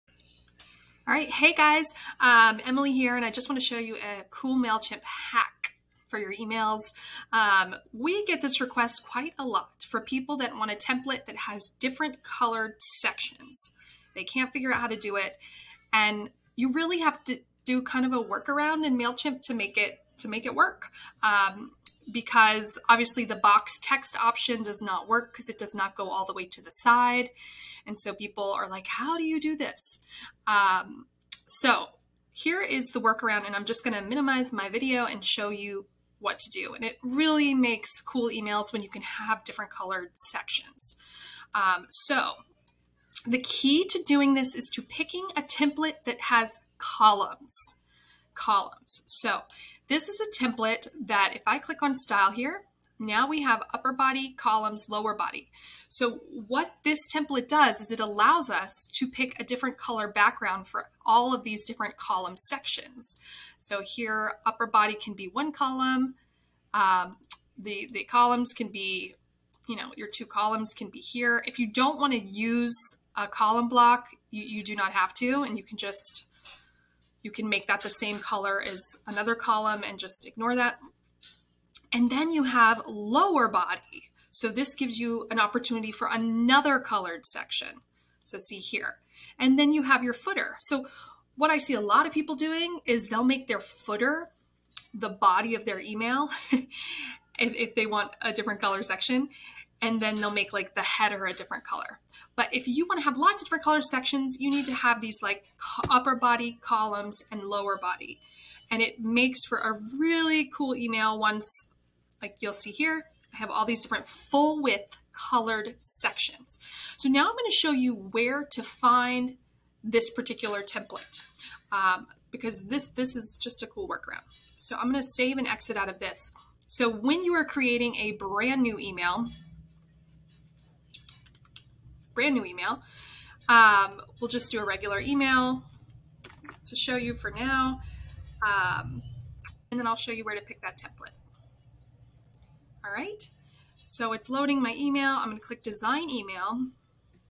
Mailchimp Hack: Different Colored Background Sections In Your Email
Learn the trick to getting different colored full-width background color sections in your email. I love this trick! It's all about picking the right template!